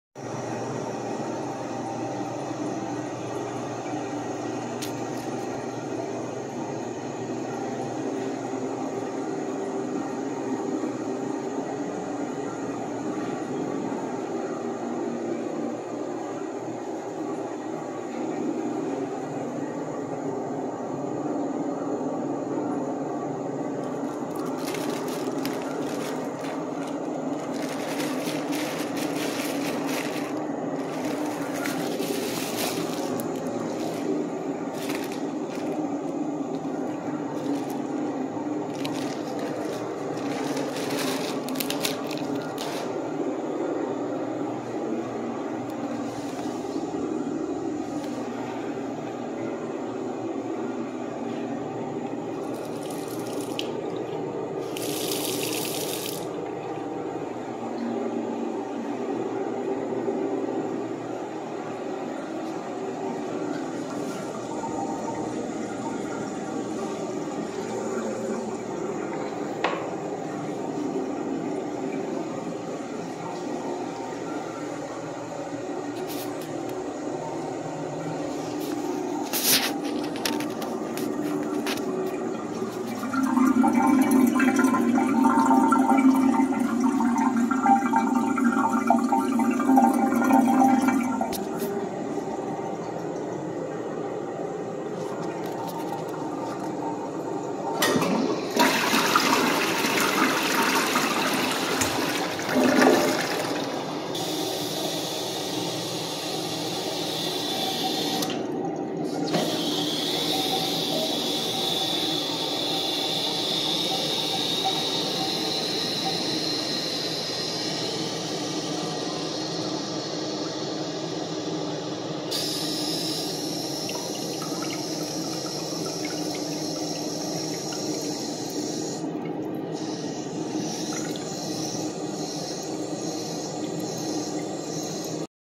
2005 Eljer Patriot Toilet
I decided to go to this place and revisit the restroom again. Everything is the same except that the Eljer Correcto is out of order. Also note how incredibly deep is the bowl on the patriot and how small the trapway is. Rating: 5/10